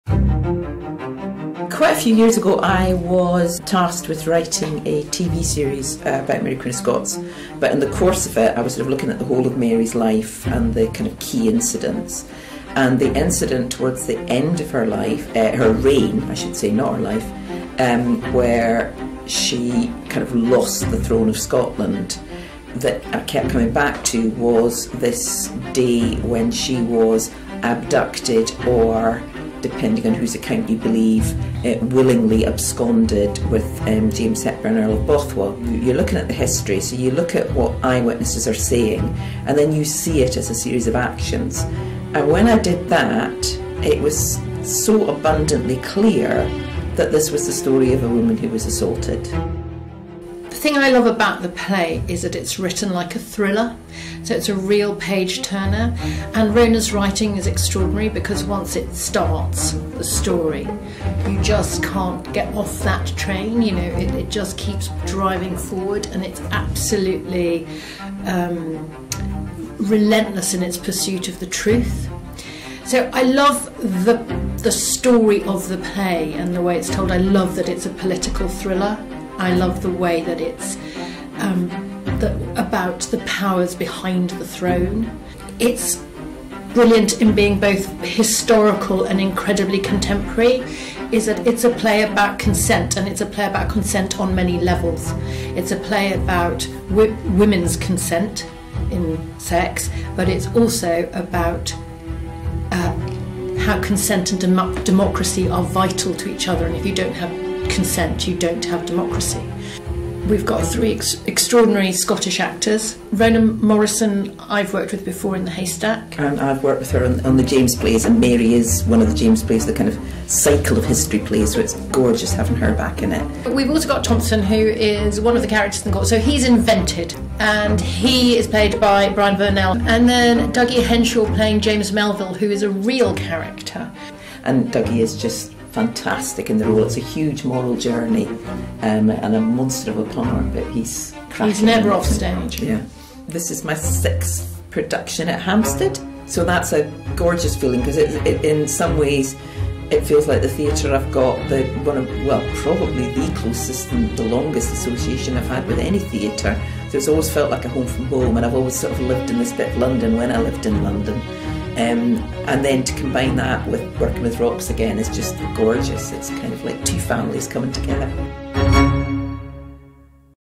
Mary interview with Rona Munro (Writer) and Roxana Silbert (Director)
Writer Rona Munro and Director Roxana Silbert introduce their new political thriller. Mary, starring Douglas Henshall (Shetland, BBC), Rona Morison (The Haystack, Hampstead Theatre) and Brian Vernel (Stories, National Theatre) plays on the Main Stage from 26 November.